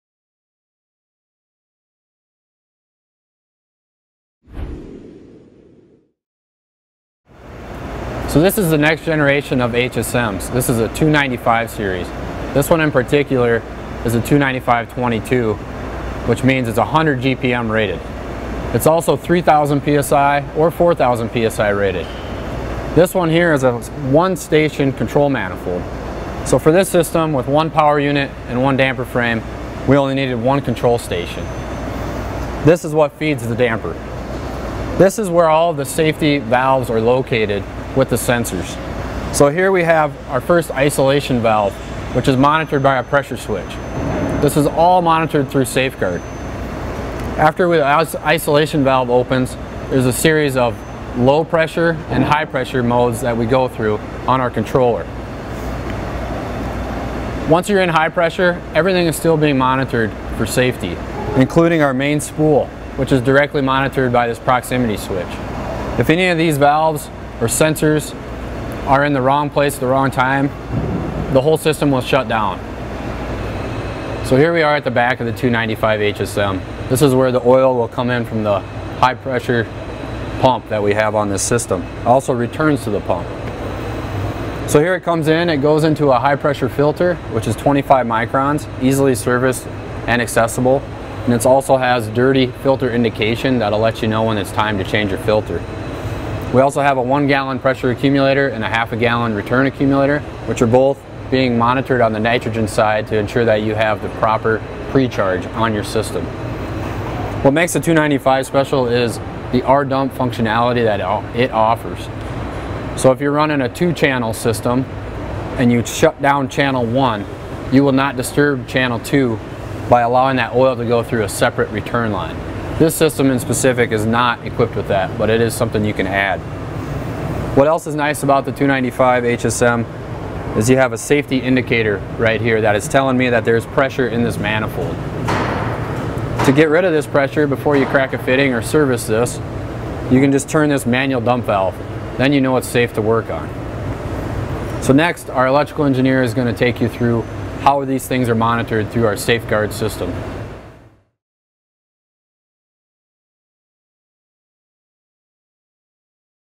MTS Series 295 HSM with SafeGuard Technology
MTS Application Engineer, Chris Carlson, describes features of the Series 295 HSM, including the SafeGuard technology interface.